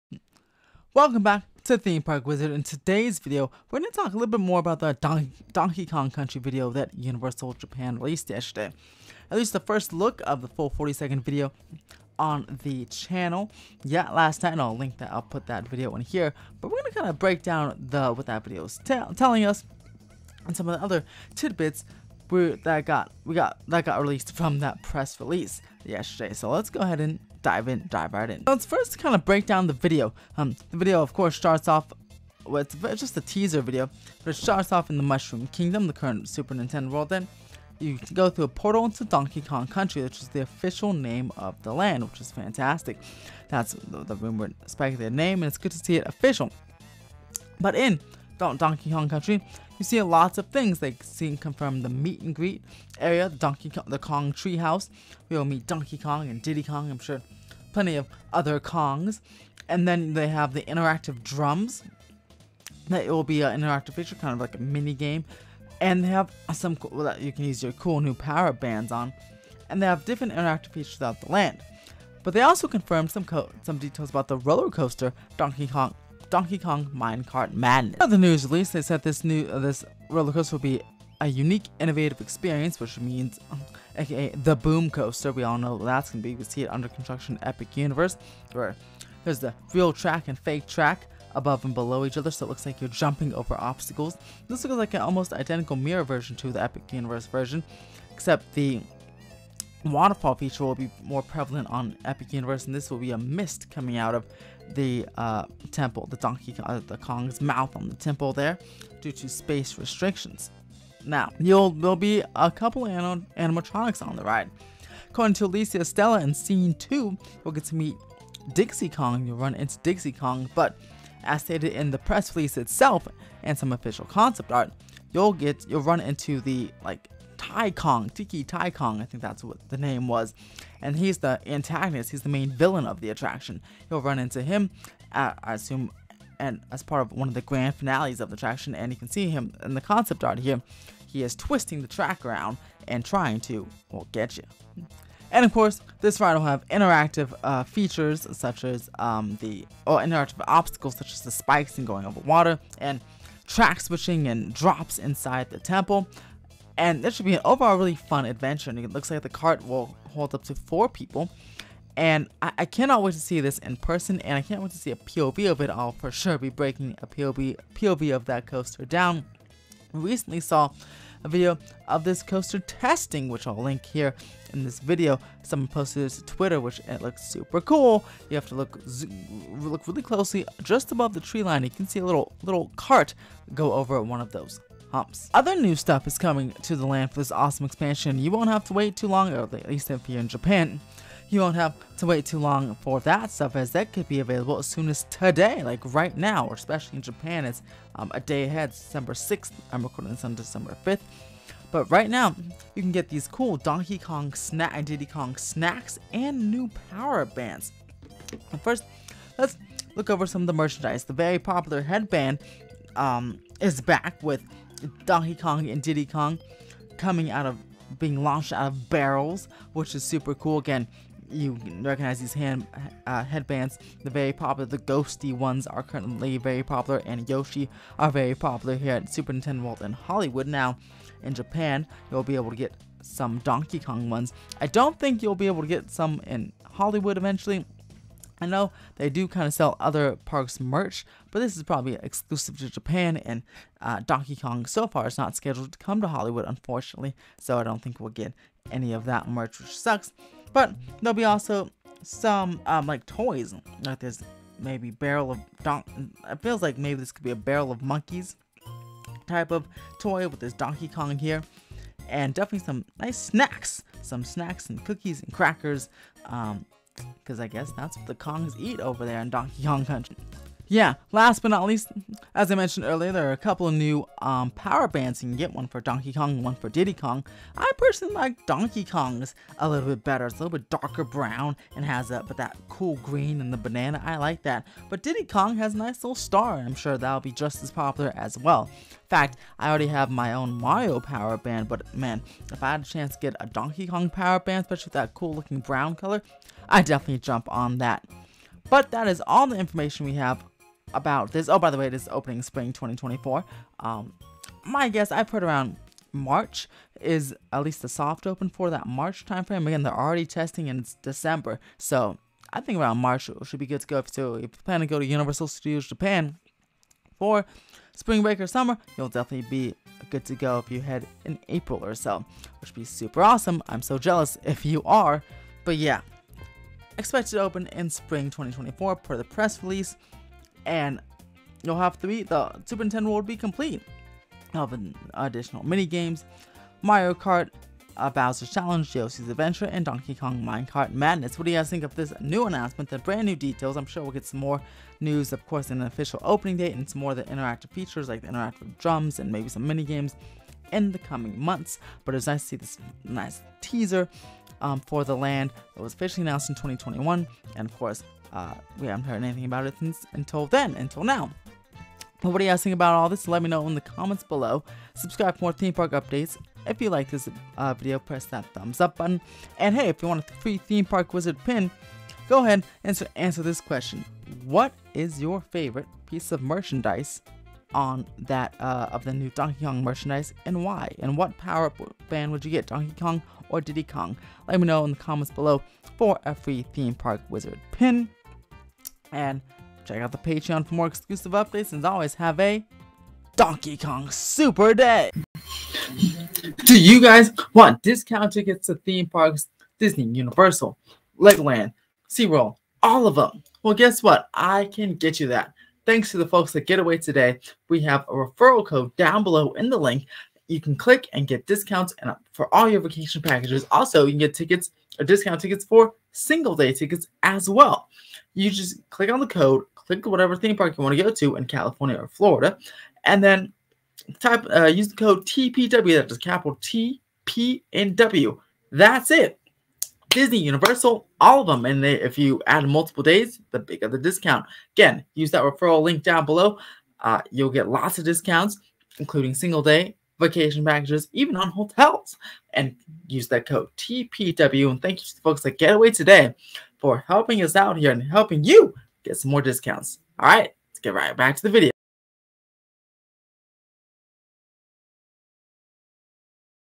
NEW DETAILS Revealed For Donkey Kong Country Opening Spring 2024! | Universal Studios Japan
Here are all the amazing games and activities coming to donkeykongcountry at universalstudiosjapan in 2024!